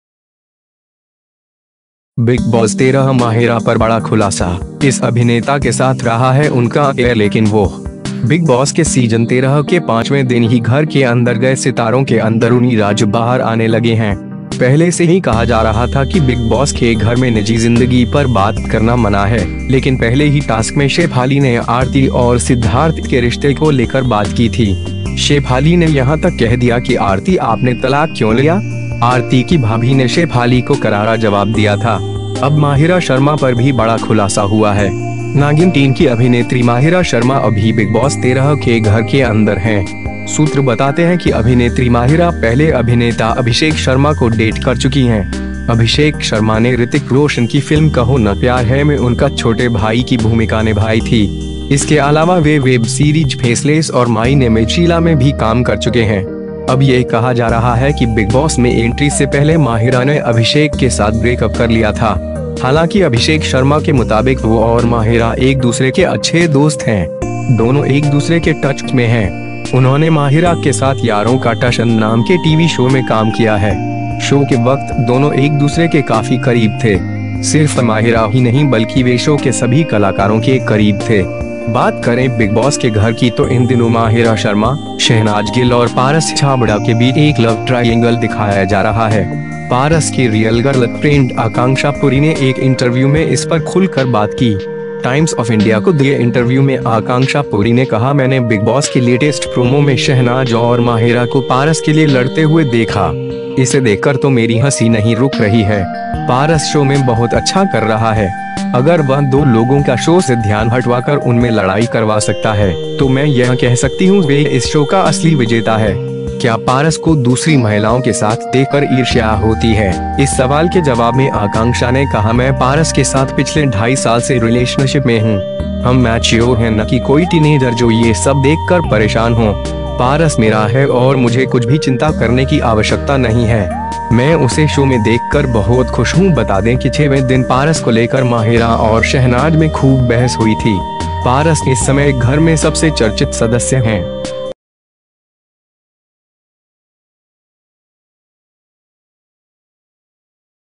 Bigg Boss 13 माहिरा पर बड़ा खुलासा, इस अभिनेता के साथ रहा है उनका अफेयर लेकिन वो
for mouth watering Indian Food Recipes Bigg Boss 13 माहिरा पर बड़ा खुलासा, इस अभिनेता के साथ रहा है उनका अफेयर लेकिन वो..

बिग बॉस के सीजन 13 के पांचवें दिन ही घर के अंदर गए सितारों के अंदरुनी राज बाहर आने लगे हैं। पहले से ही कहा जा रहा था कि बिग बॉस के घर में निजी जिंदगी पर बात करना मना है, लेकिन पहले ही टास्क में शेफाली ने आरती और सिद्धार्थ के रिश्ते को लेकर बात की थी। शेफाली ने यहां तक कह दिया कि आरती आपने तलाक क्यों लिया? आरती की भाभी ने शेफाली को करारा जवाब दिया था। अब माहिरा शर्मा पर भी बड़ा खुलासा हुआ है।

नागिन-3 की अभिनेत्री माहिरा शर्मा अभी बिग बॉस 13 के घर के अंदर हैं। सूत्र बताते हैं कि अभिनेत्री माहिरा पहले अभिनेता अभिषेक शर्मा को डेट कर चुकी हैं। अभिषेक शर्मा ने ऋतिक रोशन की फिल्म ‘कहो न प्यार है’ में उनकछोटे भाई की भूमिका निभाई थी। इसके अलावा वे वेब सीरीज फेसलेस और माई नेम इज शीला में भी काम कर चुके हैं। अब ये कहा जा रहा है कि बिग बॉस में एंट्री से पहले माहिरा ने अभिषेक के साथ ब्रेकअप कर लिया था।

हालांकि अभिषेक शर्मा के मुताबिक वो और माहिरा एक दूसरे के अच्छे दोस्त हैं। दोनों एक दूसरे के टच में हैं। उन्होंने माहिरा के साथ यारों का टशन नाम के टीवी शो में काम किया है। शो के वक्त दोनों एक दूसरे के काफी करीब थे। सिर्फ माहिरा ही नहीं बल्कि वे शो के सभी कलाकारों के करीब थे। बात करें बिग बॉस के घर की तो इन दिनों माहिरा शर्मा, शहनाज गिल और पारस छाबड़ा के बीच एक लव ट्राइएंगल दिखाया जा रहा है।

पारस की रियल गर्लफ्रेंड आकांक्षा पुरी ने एक इंटरव्यू में इस पर खुलकर बात की। टाइम्स ऑफ इंडिया को दिए इंटरव्यू में आकांक्षा पुरी ने कहा- 'मैंने बिग बॉस के लेटेस्ट प्रोमो में शहनाज और माहिरा को पारस के लिए लड़ते हुए देखा। इसे देखकर तो मेरी हंसी नहीं रुक रही है। पारस शो में बहुत अच्छा कर रहा है। अगर वह दो लोगों का शो से ध्यान हटवाकर उनमें लड़ाई करवा सकता है, तो मैं यह कह सकती हूं वह इस शो का असली विजेता है।'

क्या पारस को दूसरी महिलाओं के साथ देखकर ईर्ष्या होती है?, इस सवाल के जवाब में आकांक्षा ने कहा- 'मैं पारस के साथ पिछले ढाई साल से रिलेशनशिप में हूं। हम मैच्योर हैं न कि कोई टीनेजर जो ये सब देखकर परेशान हों। पारस मेरा है और मुझे कुछ भी चिंता करने की आवश्यकता नहीं है। मैं उसे शो में देखकर बहुत खुश हूं।' बता दें कि 6वें दिन पारस को लेकर माहिरा और शहनाज में खूब बहस हुई थी। पारस इस समय घर में सबसे चर्चित सदस्य हैं।